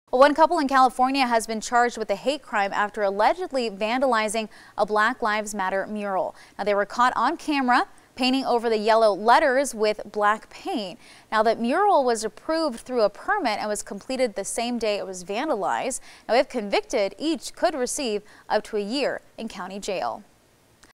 Couple in California charged with hate crime
One couple in California has been charged with a hate crime allegedly vandalizing a Black Lives Matter mural. They were caught on camera painting over the yellow letters with black paint.